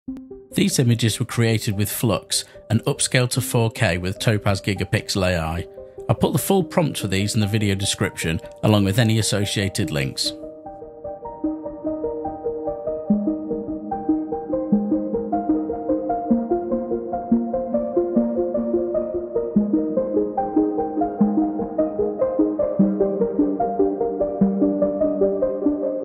Stunning 4k images with FLUX AI and Topaz Gigapixel - PROMPT INCLUDED [001]
Using using Flux AI & Topaz Gigapixel to achieve these stunning results.

Flux (Dev) Prompt:
close-up photo of a porcelain skinned woman with short pixie style blonde hair, a face painted in a striking gold color. The gold paint is cracked and chipped, The woman's eyes are wide open, and her lips are slightly open, giving her a pensive look. The background is out of focus golden material that sparkles.